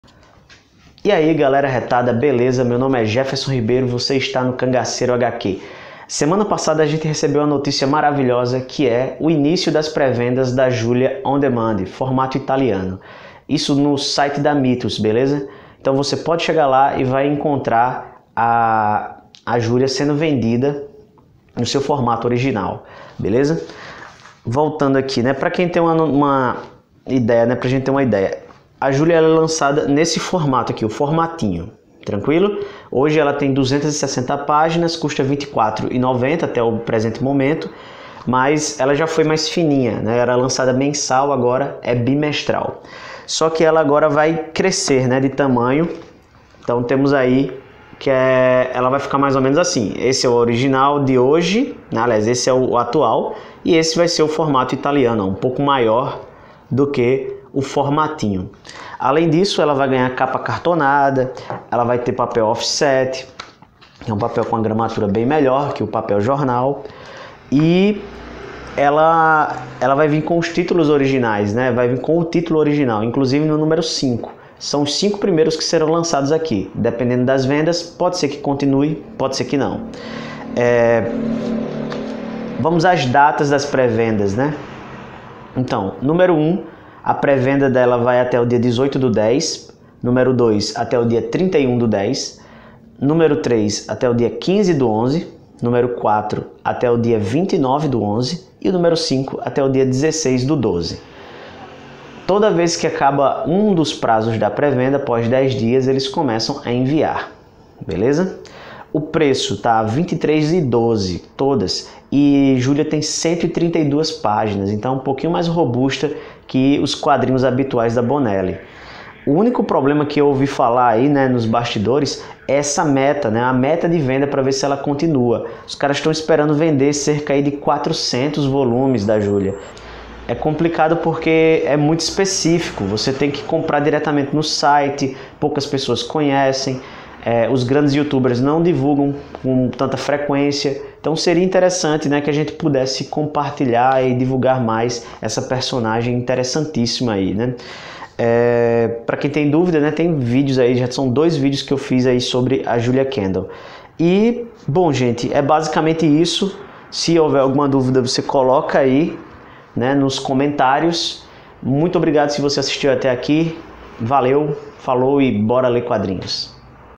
JÚLIA KENDALL: PRÉ-VENDA ON DEMAND
E AÍ GALERA ARRETADA!
Hoje venho dar informações sobre a pré-venda da Júlia Kendall On Demand.
Espero que possam descobrir e acompanhar esta incrível personagem.